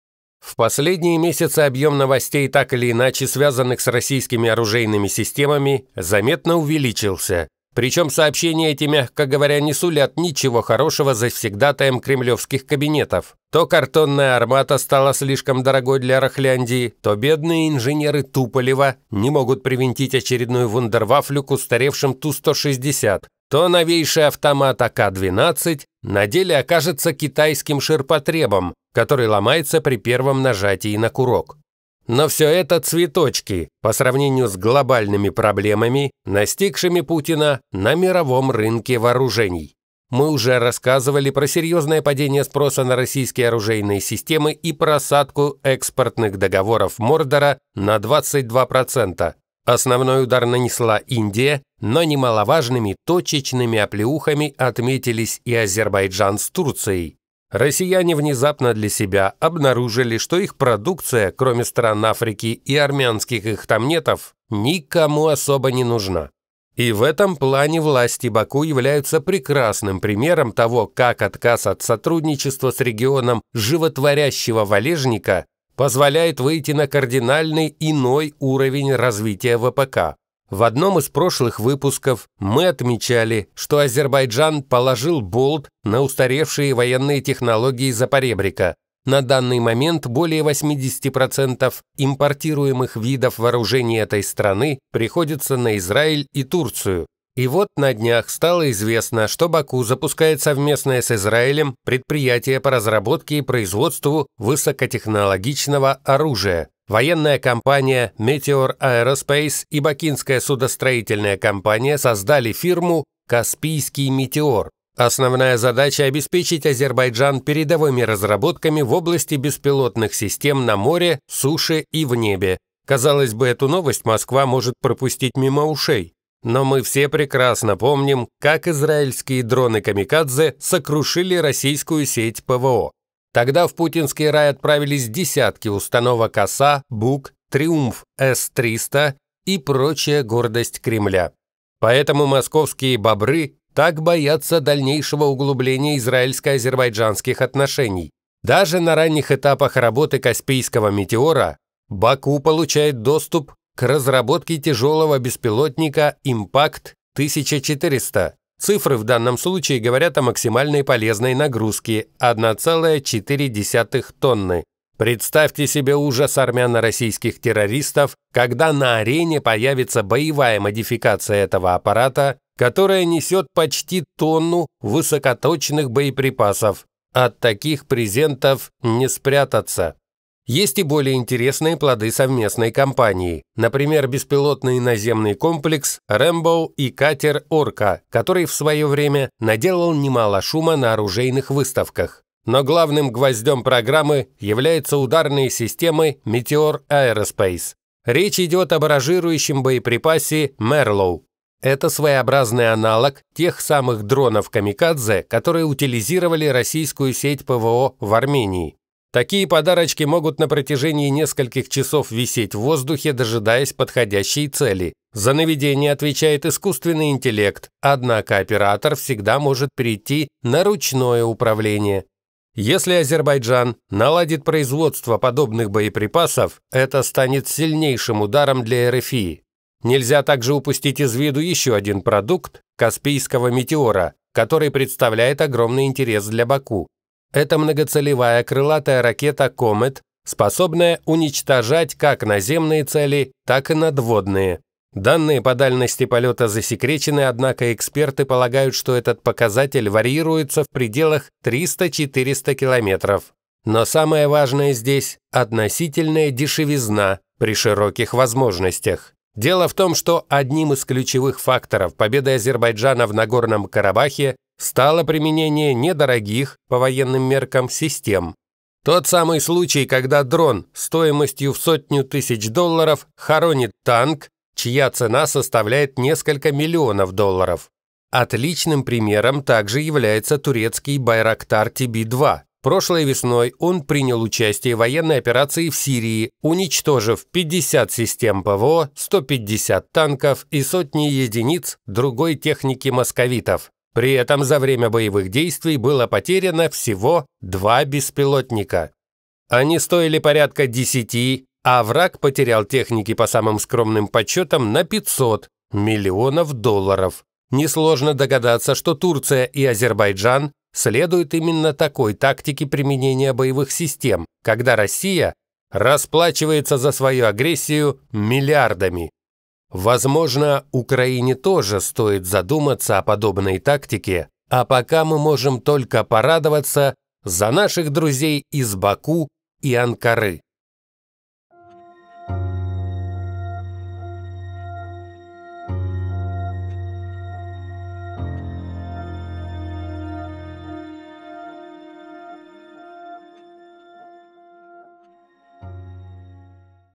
Израиль и Азербайджан разрывают ВПК Путина и запускают совместное производство оружия
Автор: Павел Кот
Серьезное падение спроса на российские оружейные системы и просадка экспортных договоров Мордора на 22%. Основной удар нанесла Индия, но немаловажными точечными оплеухами отметились и Азербайджан с Турцией.